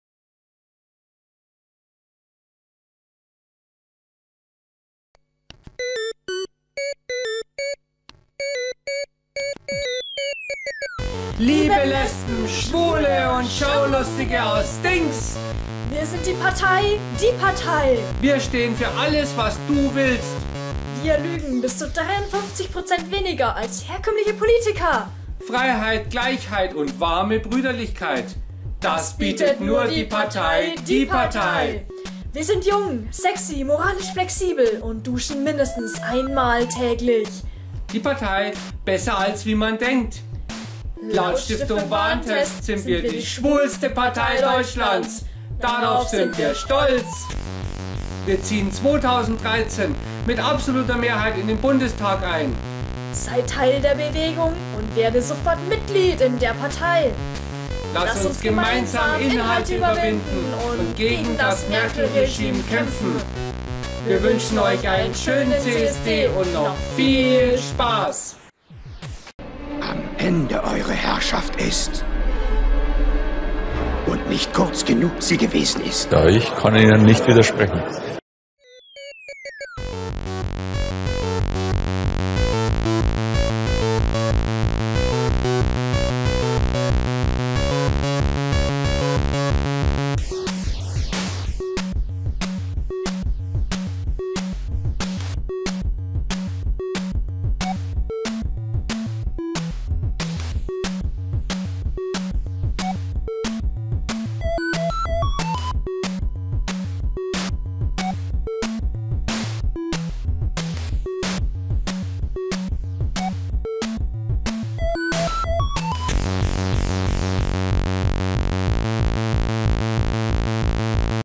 CSD Nürnberg 2012 - Die PARTEI - HD
Ein kurzes Video vom PARTEI BAYERN Lautsprecherwagen, unterlegt mit unserem Demo-Jingle. Schwule und Lesben und _ werden sofort MitgliedIn_ in der PARTEI